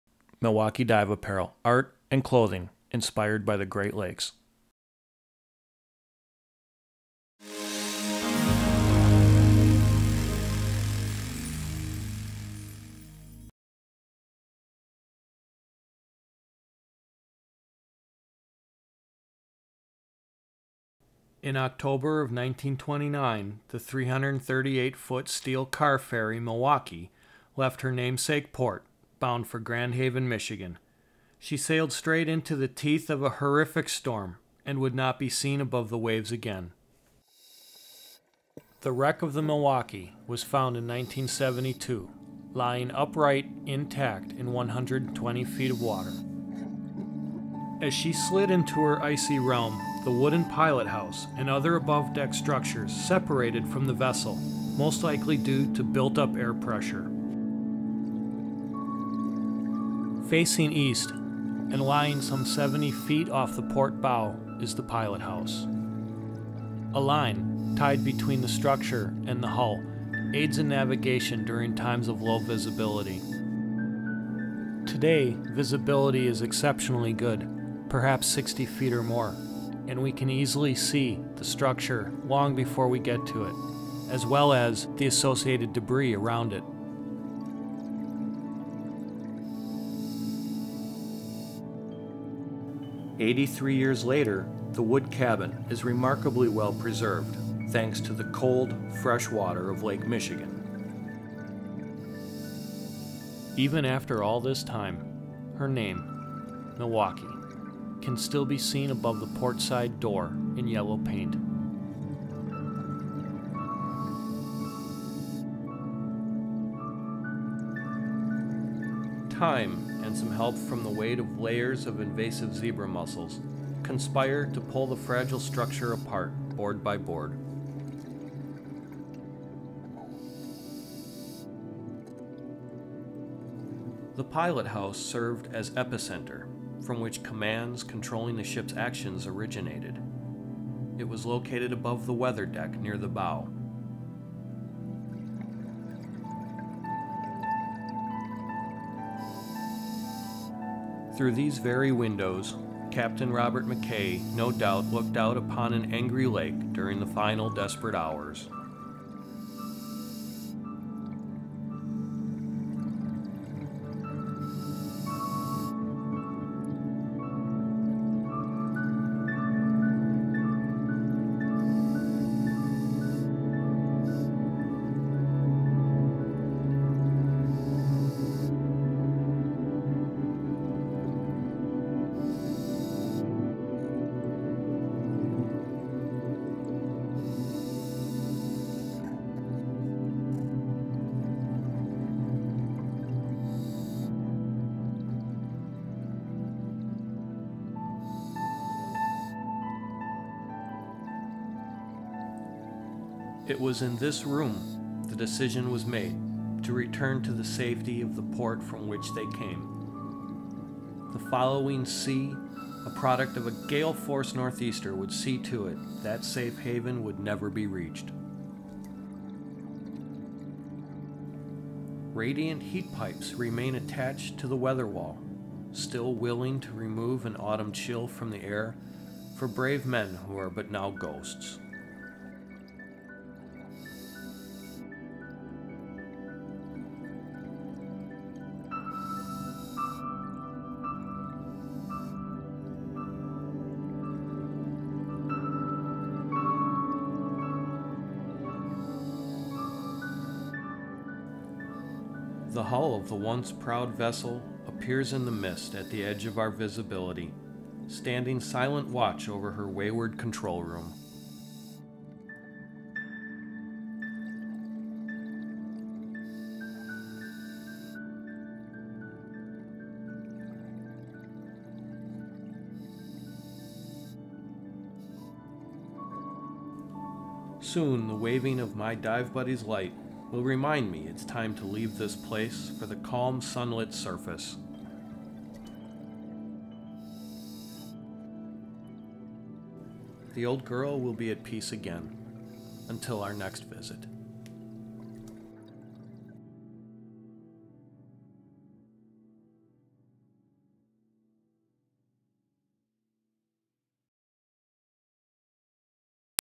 Milwaukee
The 338' steel rail car ferry Milwaukee which sank in 1929 off Fox Point, WI, is a favorite dive site of recreational and technical divers alike.  This video concentrates on the pilot house, and shows it from outside and in.  Shot in 2012 by diver Cal Kothrade.